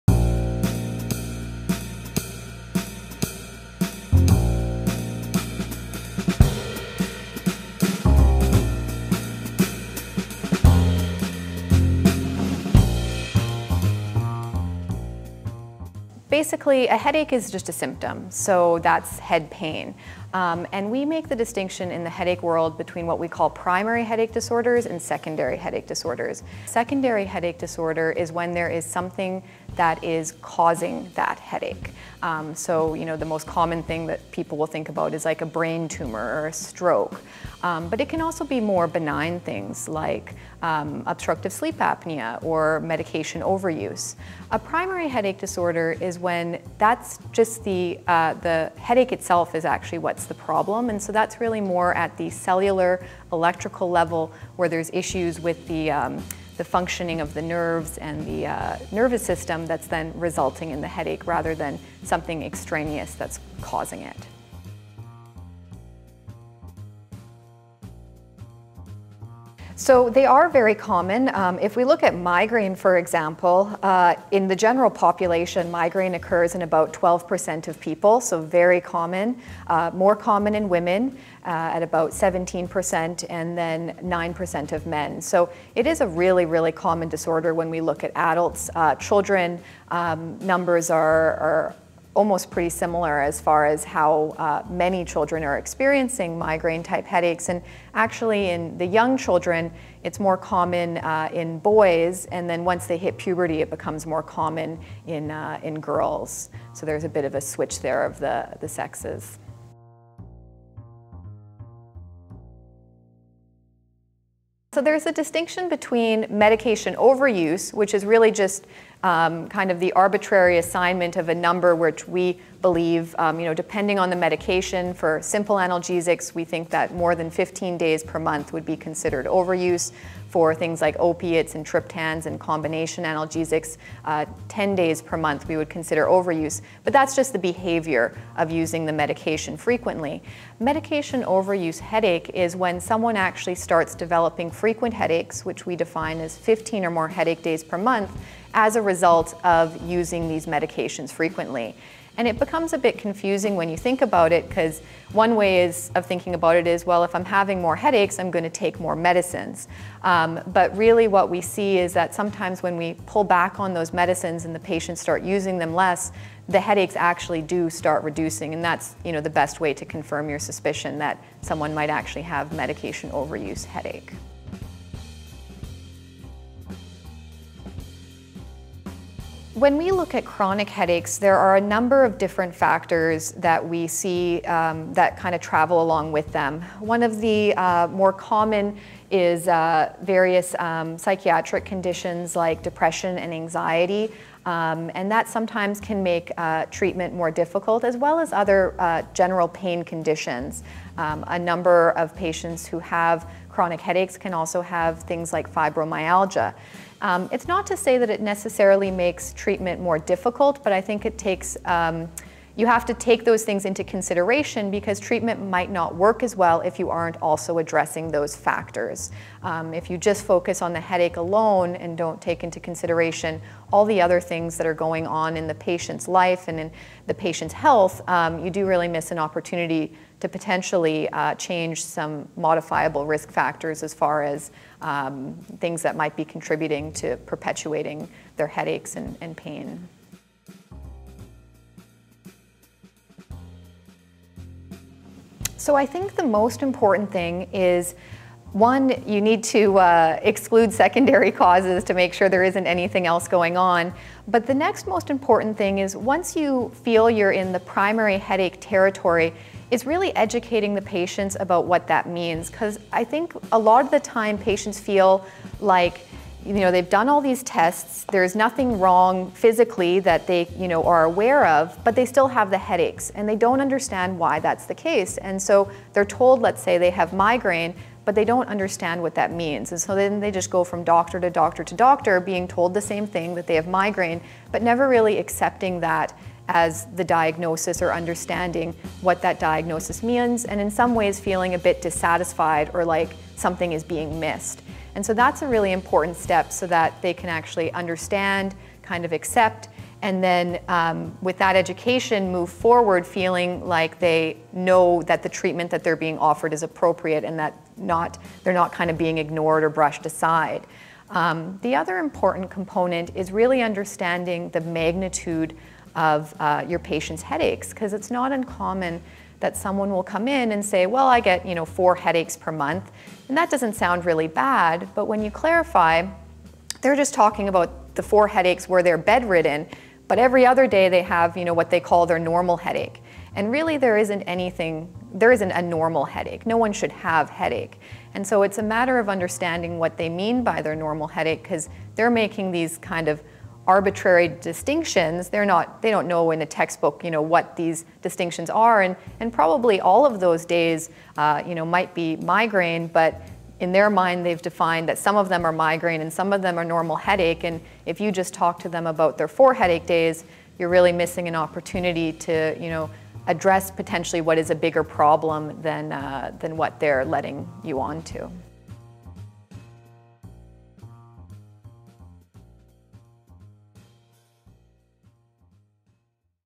Headache, Primary Headache, Secondary Headache: What’s What? It’s Giving Me a Headache!
Headaches, of all varieties, are a common occurrence in adult and pediatric populations. What’s the distinction between types, and how best can practitioners make that distinction? Dr. VanderPluym of the Mayo Clinic, a pediatric neurologist specializing in headache and concussion, discusses.